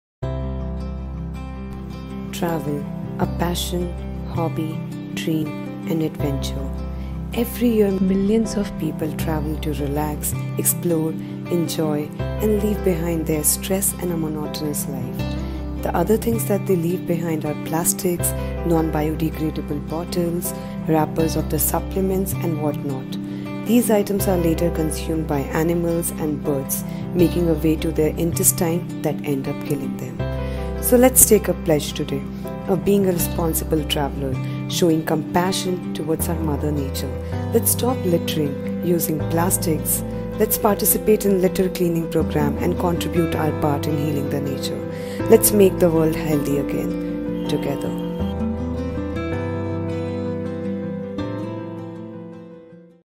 World Environment Day
It’s time to take a pledge to protect the environment, wildlife, oceans and our health, both at home and around the world. This year’s World Environment Day’s theme is “Beat plastic pollution”- remember to consciously reduce litter and use recyclable bags! Let’s support each other in leaving the world a greener and more beautiful place for our future generations.